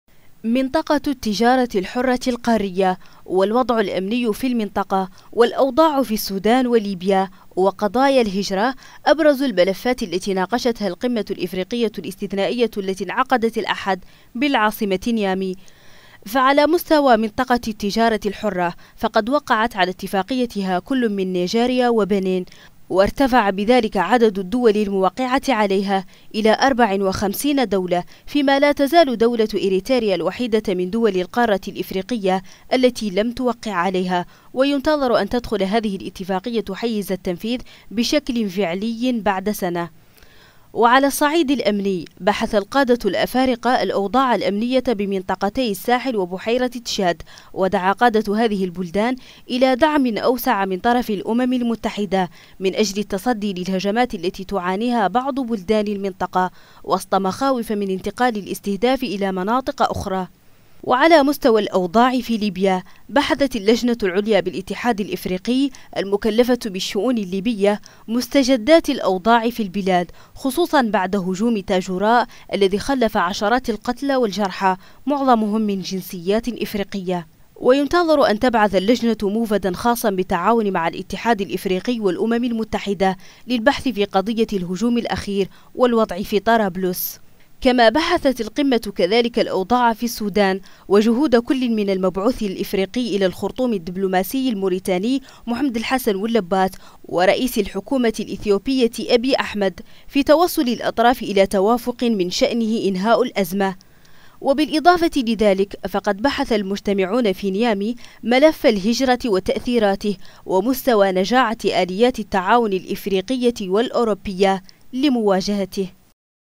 أبرز ملفات القمة الاستثنائية الإفريقية تقرير آمنة زيدان
ناقش القادة الأفارقة خلال قمة استثنائية للاتحاد الإفريقي، انعدقت الأحد بالعاصمة النيجرية نيامي، ملفات عديدة، بينها اتفاقية منطقة التجارة الحرة، التي وقع عليها خلال القمة دولتا نيجيريا وبنين.
كما بحث القادة الأفارقة كذلك، ملفات الوضع الأمني في منطقتي الساحل وبحيرة اتشاد، والأوضاع في السودان وليبيا.